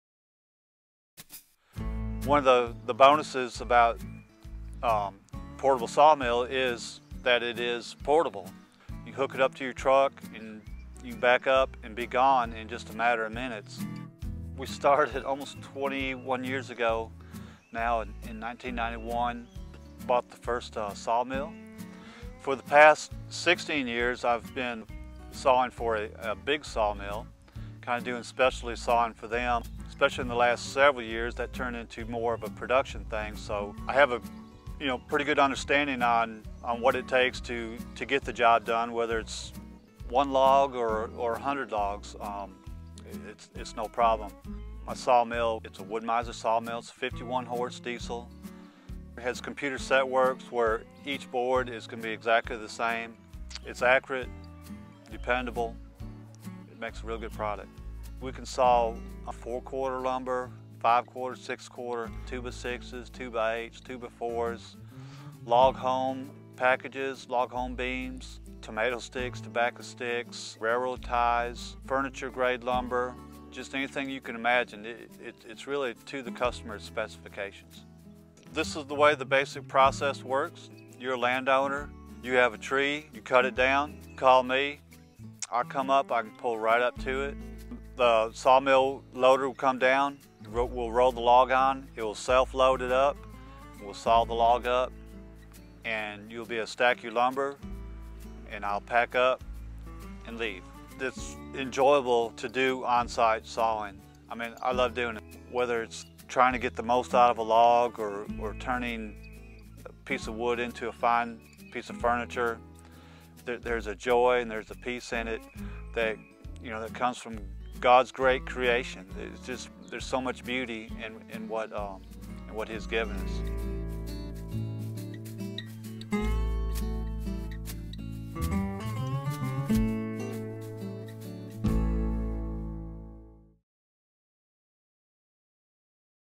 Walnut Hill portable sawmill
In this video, Walnut Hill Woodworks shows off the power and efficiency of its portable sawmill.